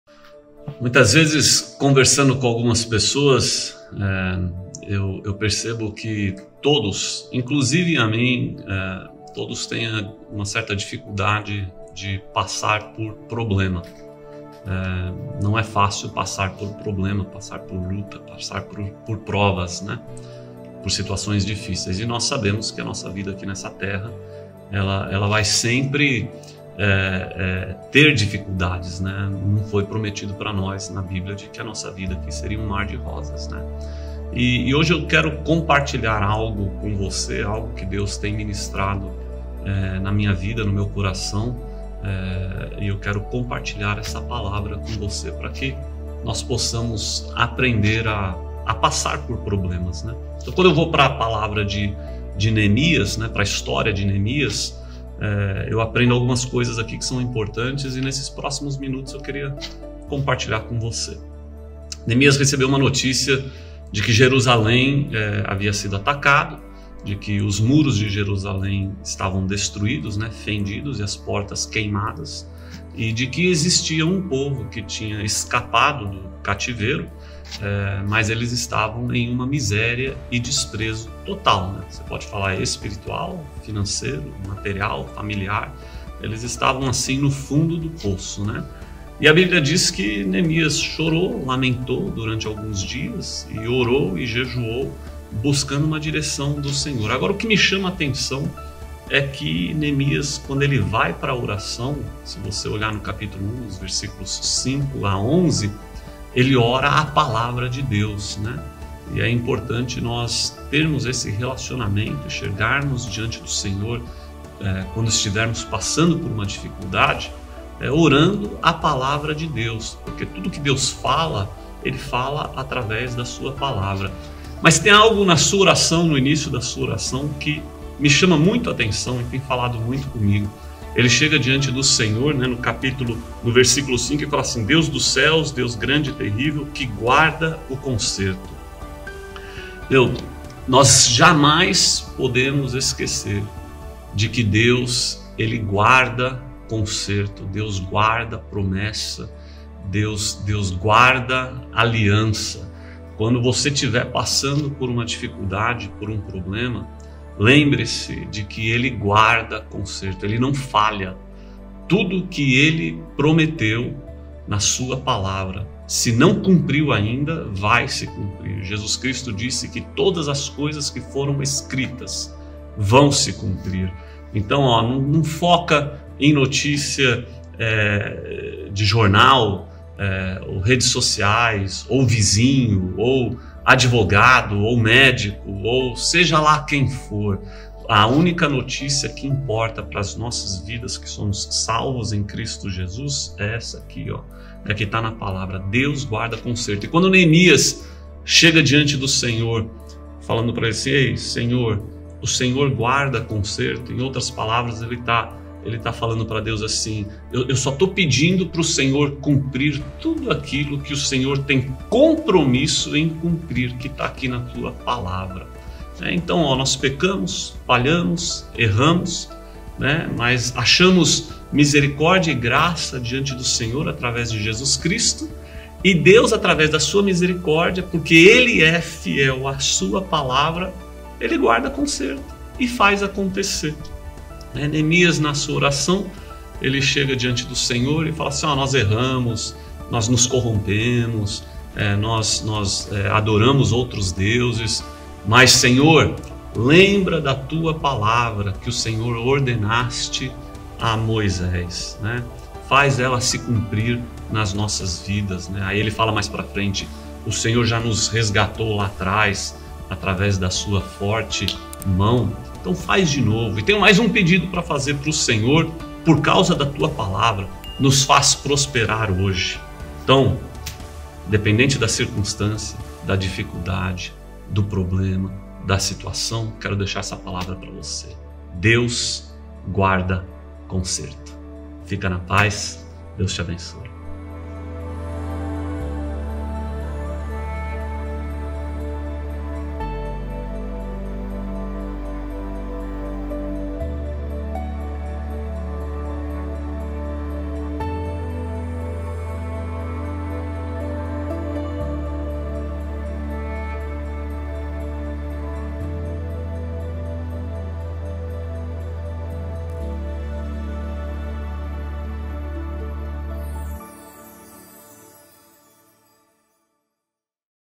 Estratégia de Neemias
O capítulo 1 do livro de Neemias nos ensina como devemos encarar uma situação difícil. Assista e seja edificado.